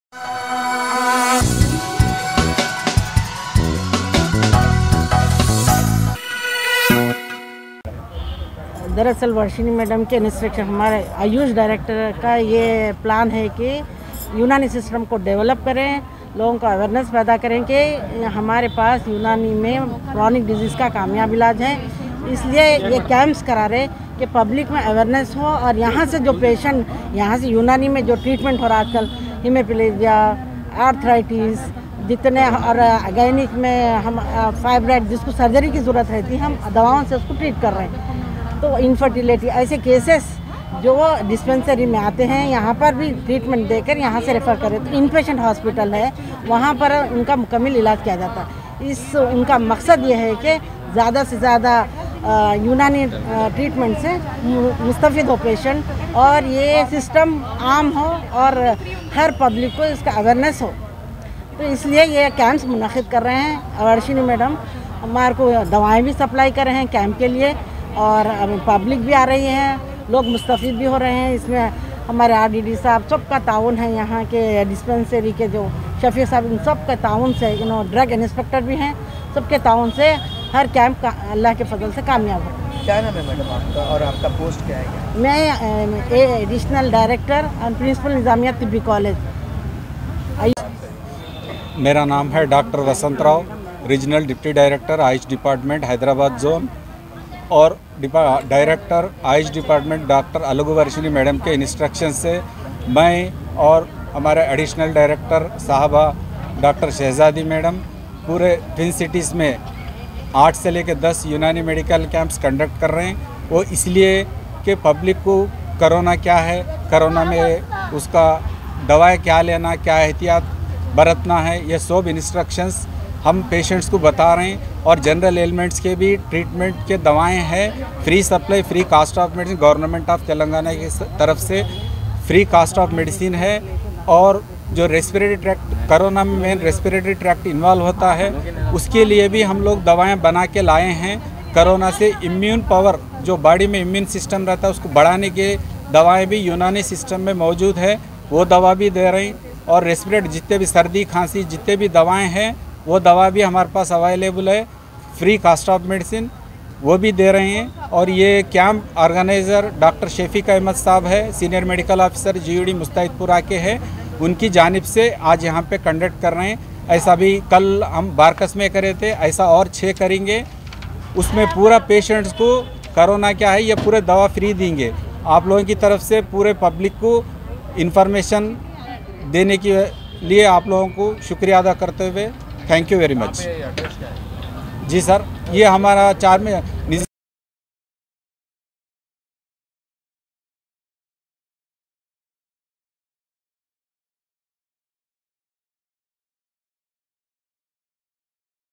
Unani Medical Camp by Government Nizamia General Hospital & Tibbi College Charminar Under Karwan Con
Unani Medical Camp by Government Nizamia General Hospital & Tibbi College Charminar Under Karwan Constituency
 कारवां निर्वाचन क्षेत्र के अंतर्गत शासकीय निजामिया सामान्य अस्पताल एवं तिब्बी कॉलेज चारमीनार द्वारा यूनानी चिकित्सा शिविर